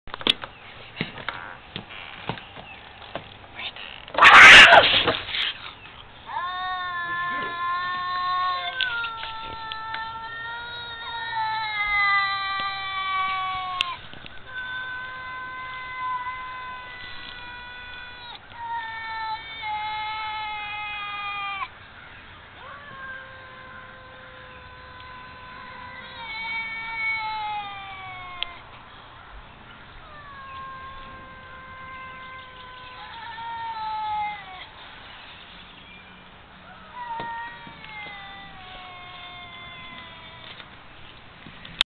Is this normal cat talk?
These are two wild cats that have lived in the woods behind our house since they were kittens.  This is the first time we've heard them "talking". Is this normal cat speak??